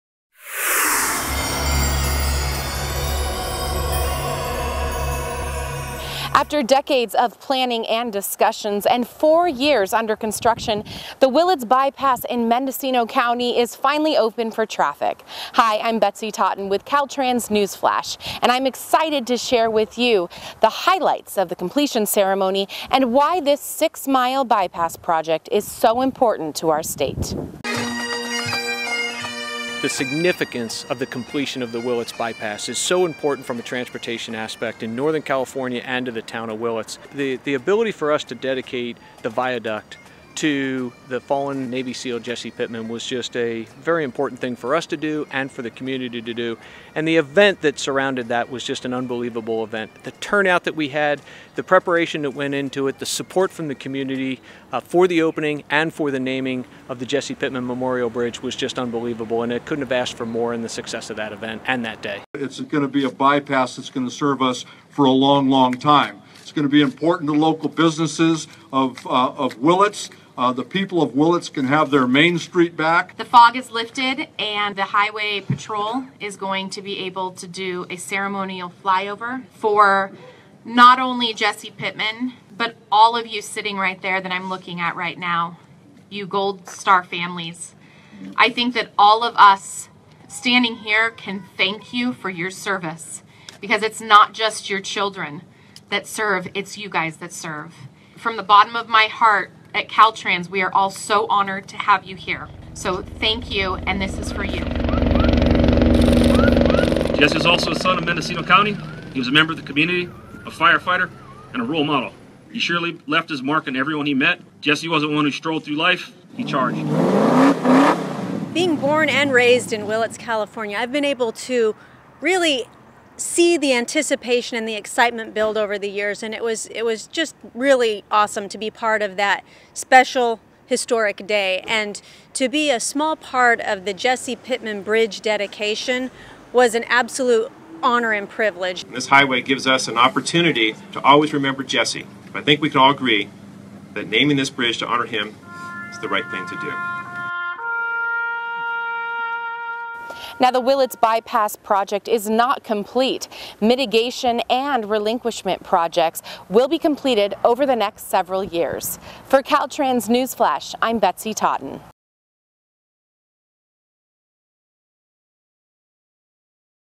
Caltrans News Flash #103 - Willits Bypass Opening
In this News Flash, you will see the highlights of the recent Willits Bypass completion celebration and dedication of the Jesse D. Pittman SO1 Navy SEAL Memorial Bridge, in honor of a fallen local hero. You will also hear about the importance of this six-mile bypass for not only the Willits area, but for the state of California.